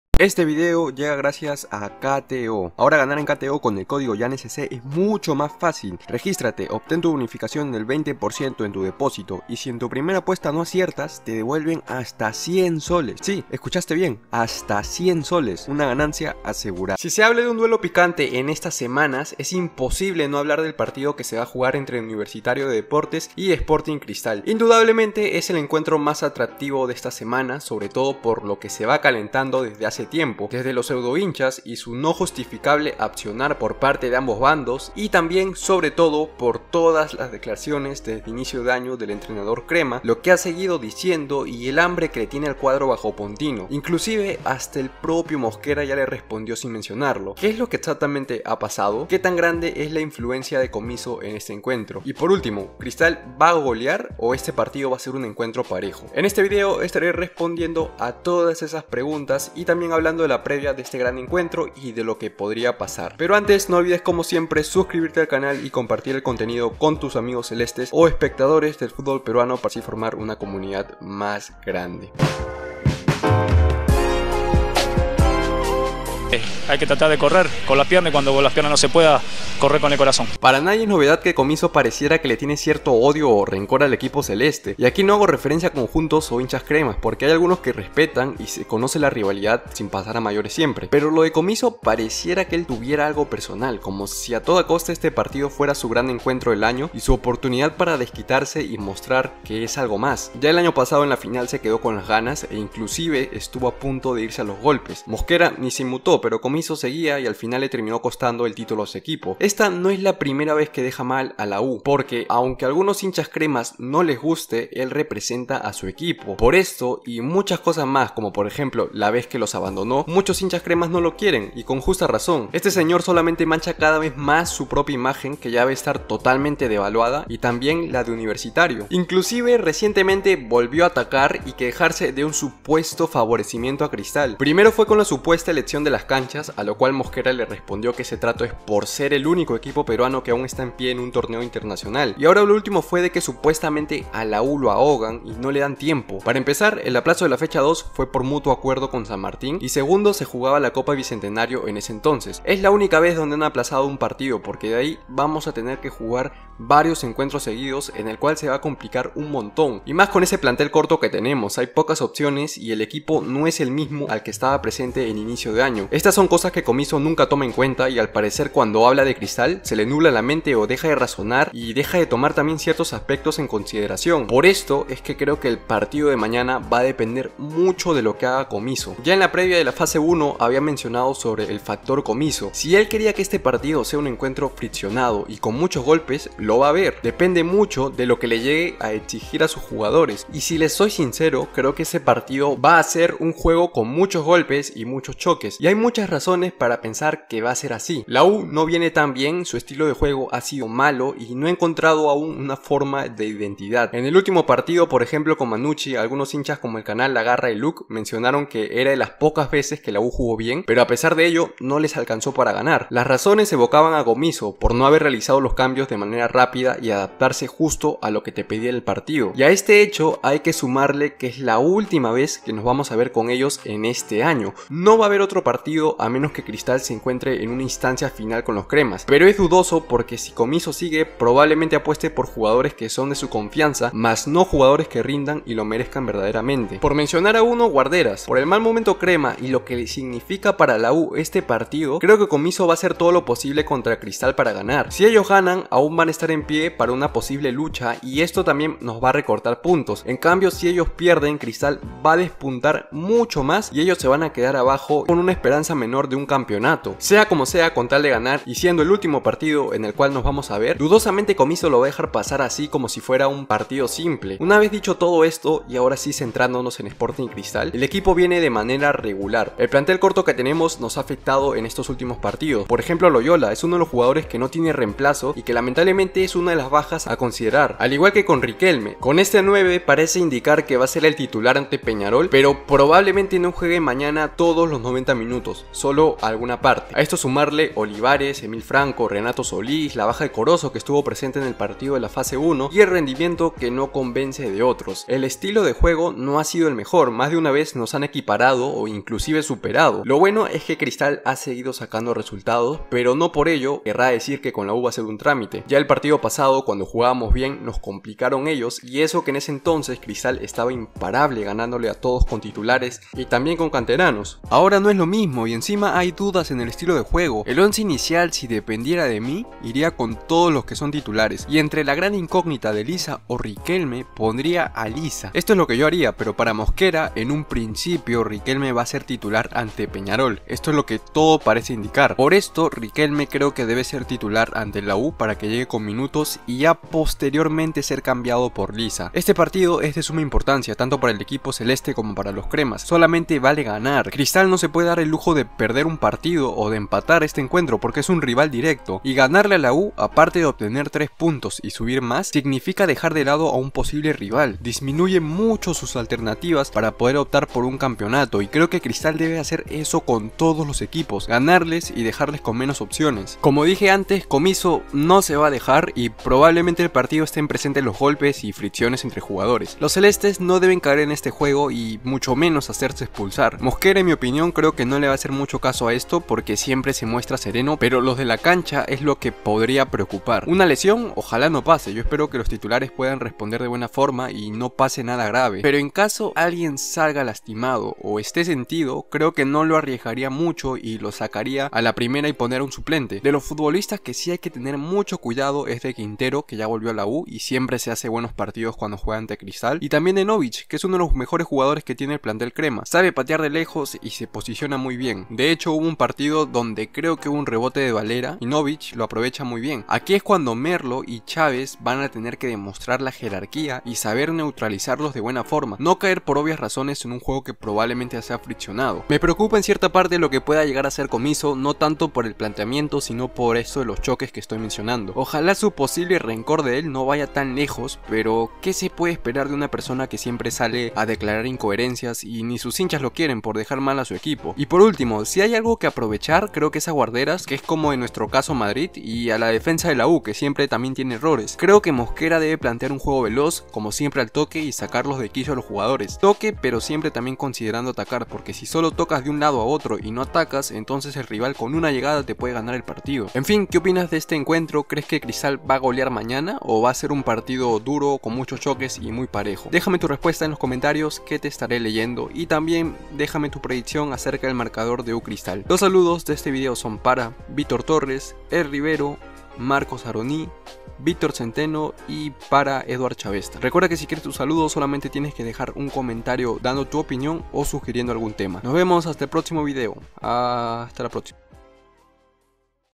DECISIVO: Sporting Cristal vs Universitario | Comizzo Ataca | ¿Habrá Goleada? | PREVIA
Sporting Cristal jugará vs Universitario de Deportes en el partido más importante de la semana. Este encuentro va a ser complicado para ambos planteles pues se juegan mucho y puede definir el camino al título para los celestes o cremas.

Código: GIANSC